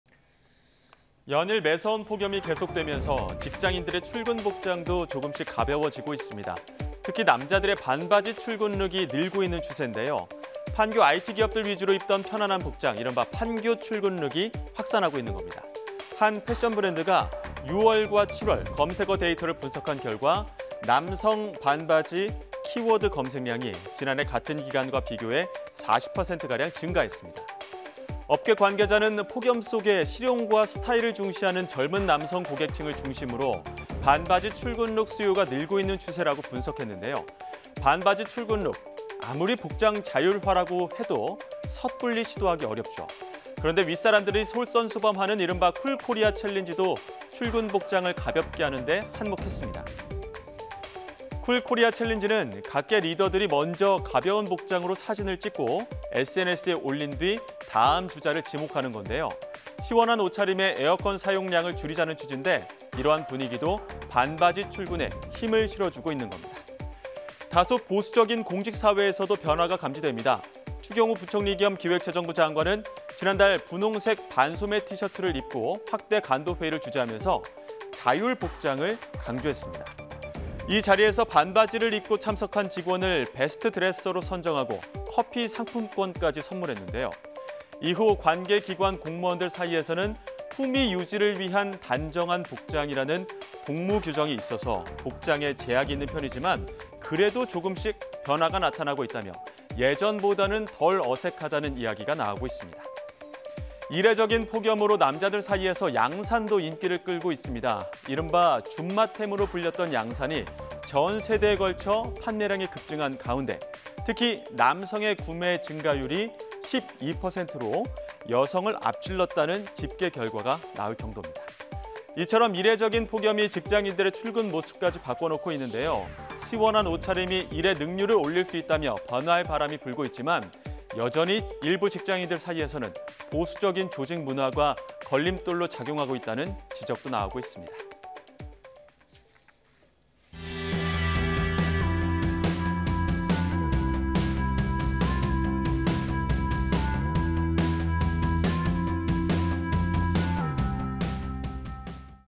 [세상만사] "반바지에 양산 쓰고 출근하는 남성들" ··· 줌마템인 양산도 인기
연일 매서운 폭염이 계속되면서
직장인들의 출근 복장도
조금씩 가벼워지고 있습니다.

특히 남자들의 반바지 출근룩이
늘고 있는 추세인데요.

 판교 IT 기업들 위주로 입던 편안한 복장,
이른바 판교 출근룩이 확산하고 있는 겁니다.

 한 패션 브랜드가
6월과 7월 검색어 데이터를 분석한 결과
'남성 반바지' 키워드 검색량이
지난해 같은 기간과 비교해 40%가량 증가했습니다